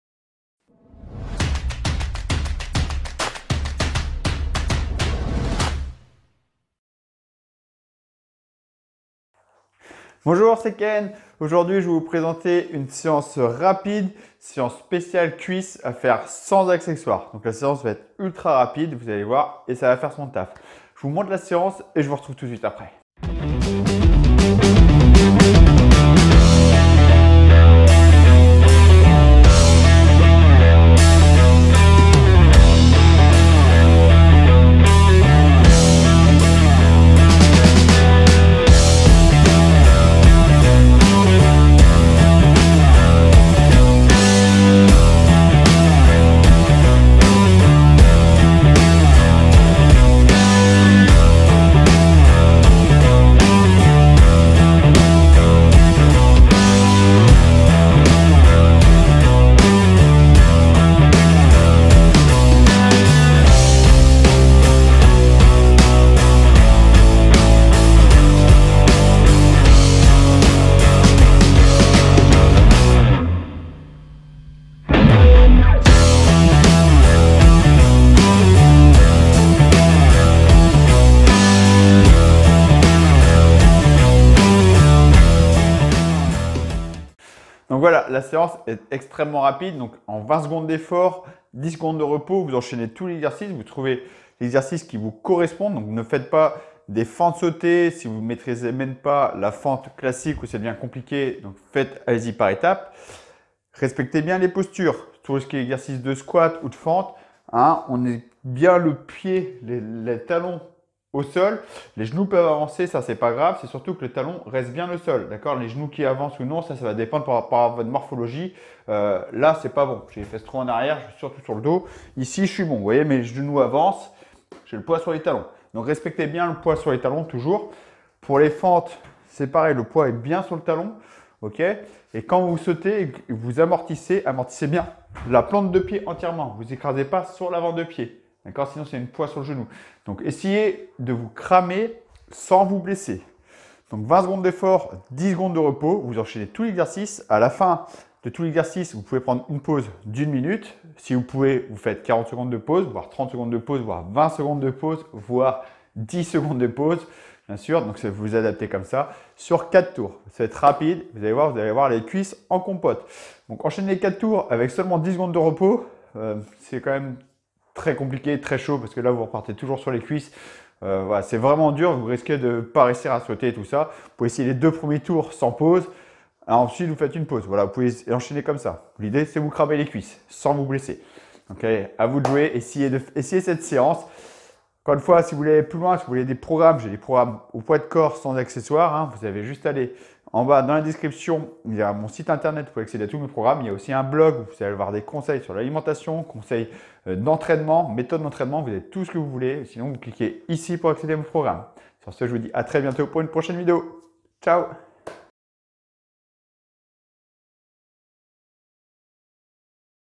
Séance jambes sans matériel
Petite séance jambes sans matériel. Voici une séance très rapide qui brûlera les jambes aussi rapidement, pas besoin d'accessoire donc pas d'excuse ;).